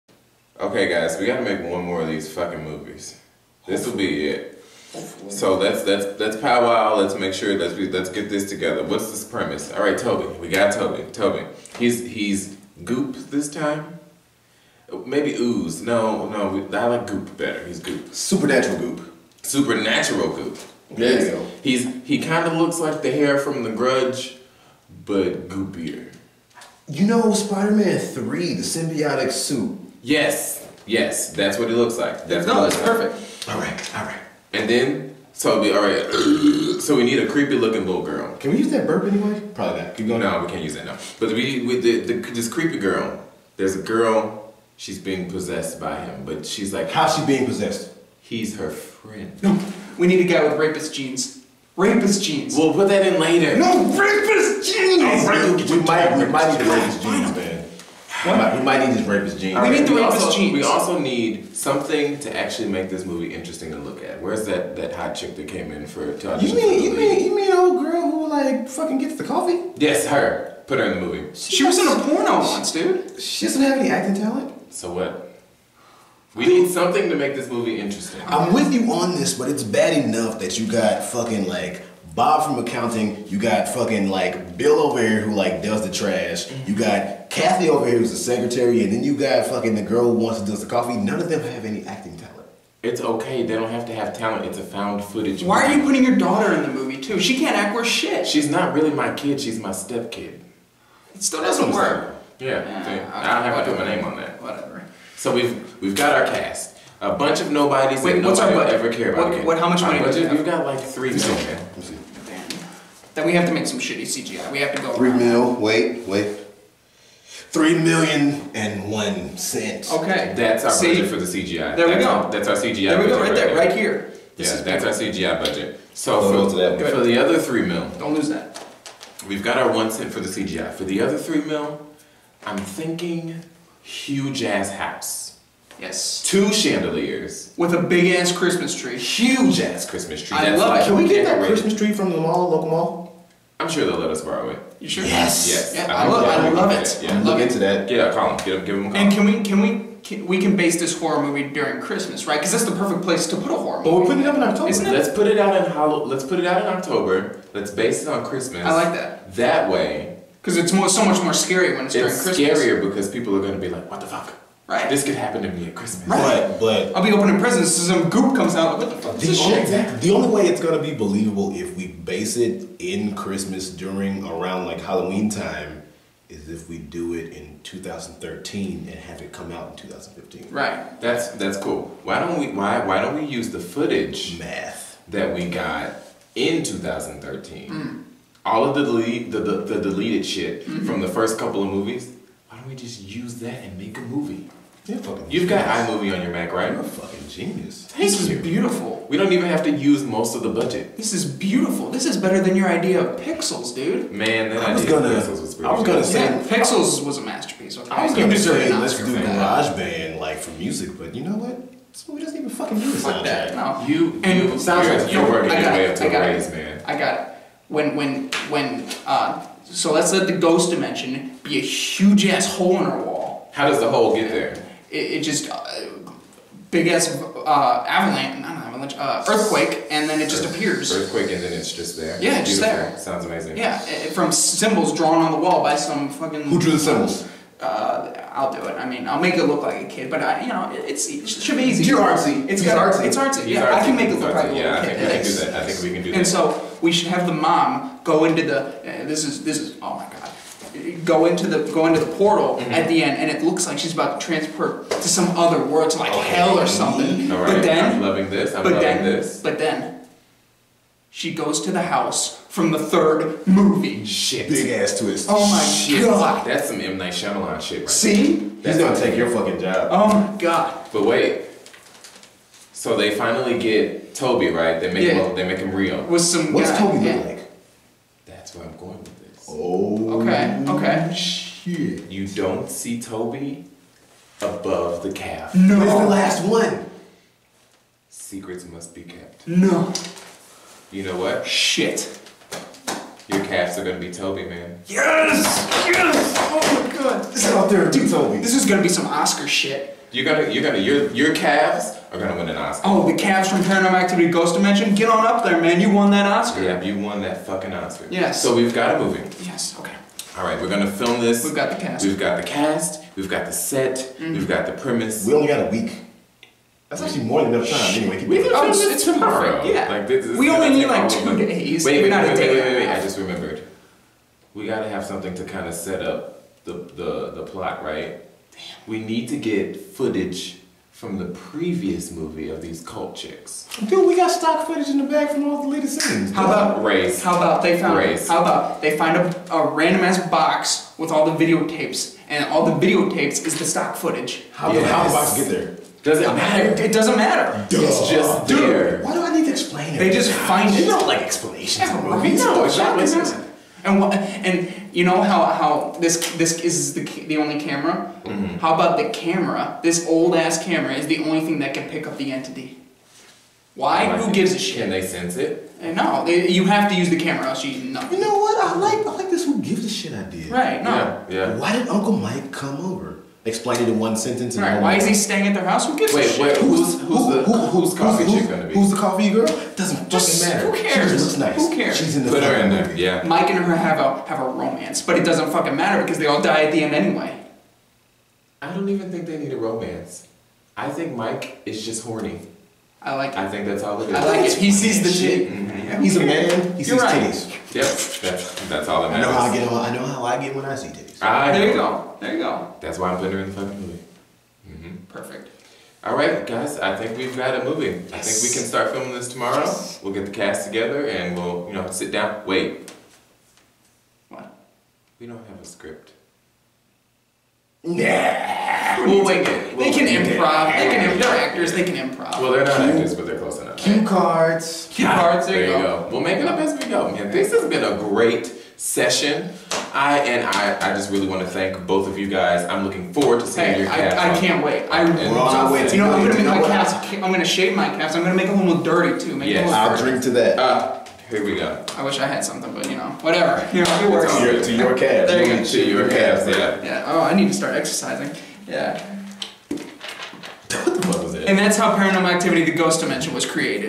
How Paranormal Activity: Ghost Dimensions Was Probably Created...
Here is a little video to tide you over till the full review comes out Sunday. Sincerest apologies for the slightly extended break! We hope this will ease your worries!